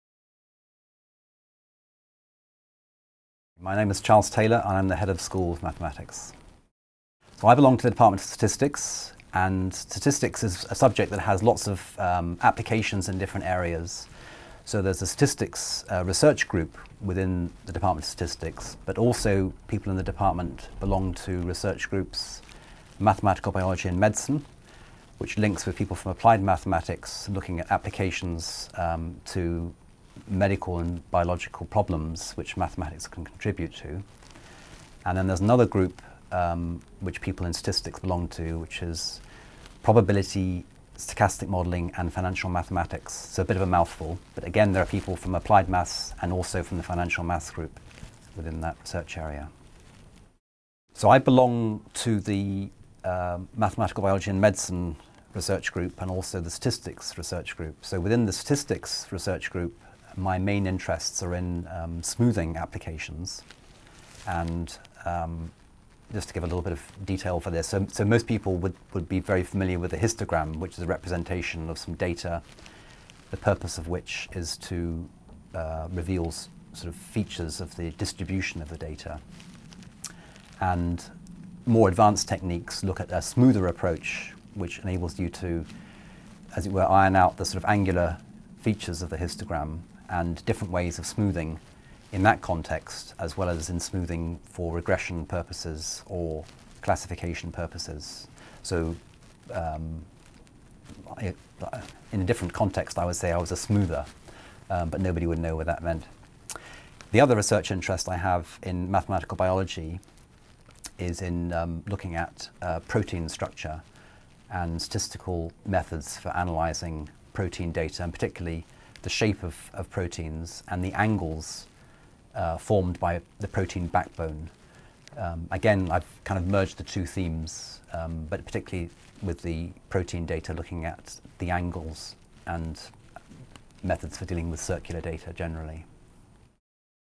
Meet the Head of Mathematics at Leeds University
A chance for you to meet Prof Charles Taylor, Head of the School of Mathematics at the University of Leeds.